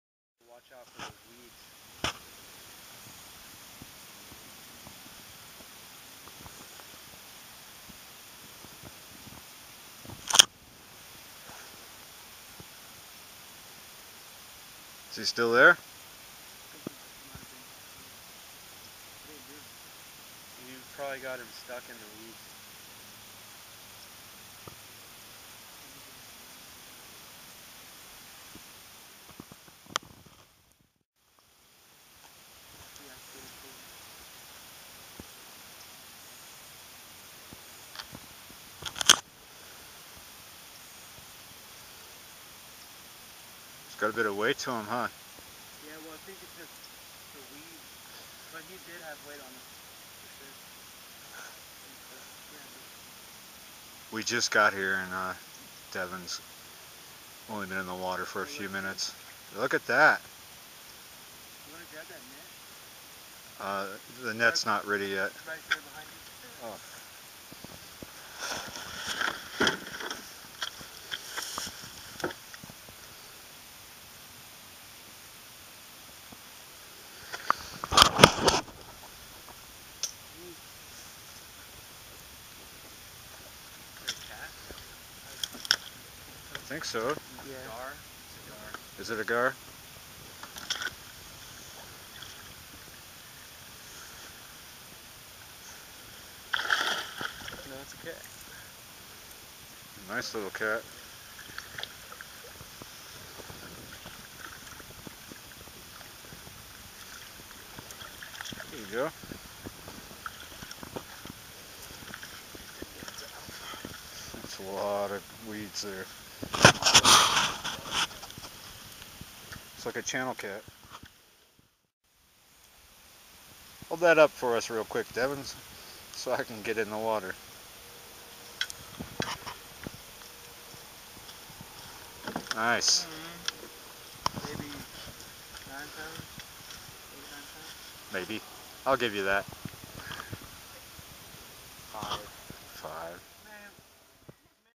Channel Catfish, Bayou Bienvenue
A nice catch from the out fall canal.